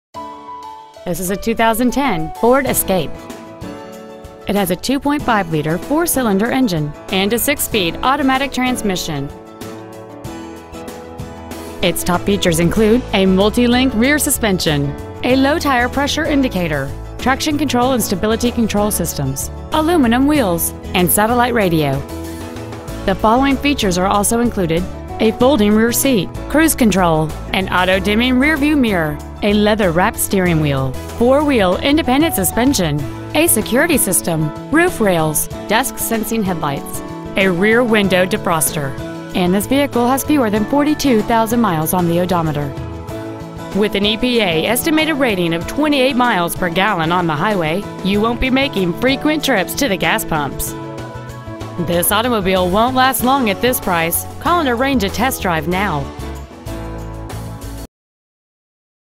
2010 FORD ESCAPE Sheffield Village OH
Year : 2010

 Make : FORD

 Model : ESCAPE

 Engine : 2.5L I4

 Trans . : AUTO 6SPD

 Exterior : BLUE

 Miles : 41,202

 5050 Detroit Road

 2010 FORD ESCAPE Sheffield Village  OH